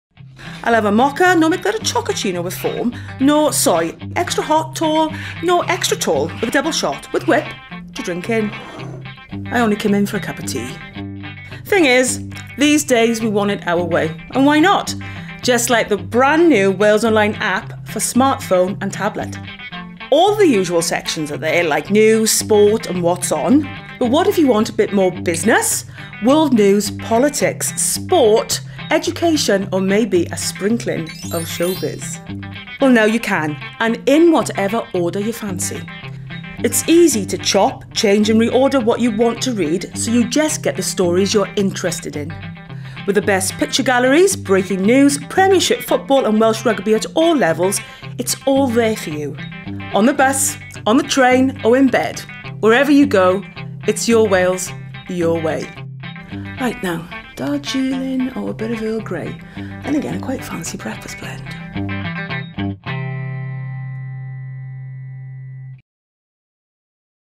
Get the new WalesOnline App - for iPad, iPhone and Android devices
Your Wales, Your way.  All the news from WalesOnline on your phone and tablet. Available from Google Play or the App Store.